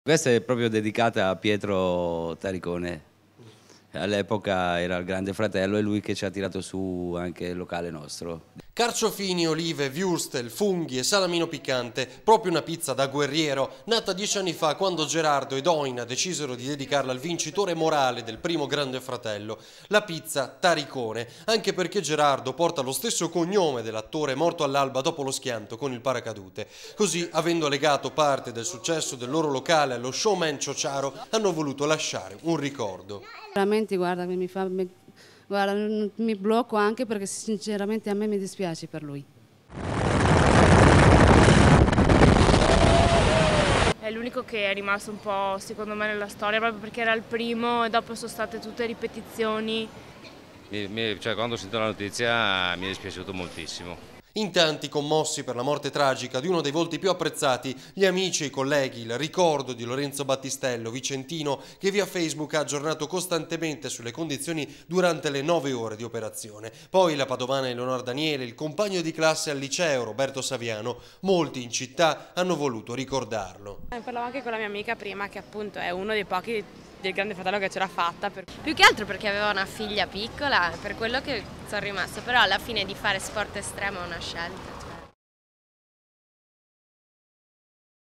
TARICONE: SHOCK A PADOVA E UNA PIZZA IN SUO ONORE
Shock per la morte di Pietro Taricone, l'attore schiantatosi con il paracadute. Il ricordo di tanti in città, anche di un pizzaiolo che porta il suo stesso cognome e che ha deciso di dedicargli una pizza. Il servizio di TG Padova di Rete Veneta